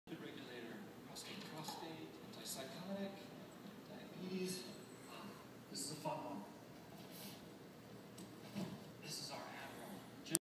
Pumpkin fetching hair ties
Our cat fetches elastic hair ties.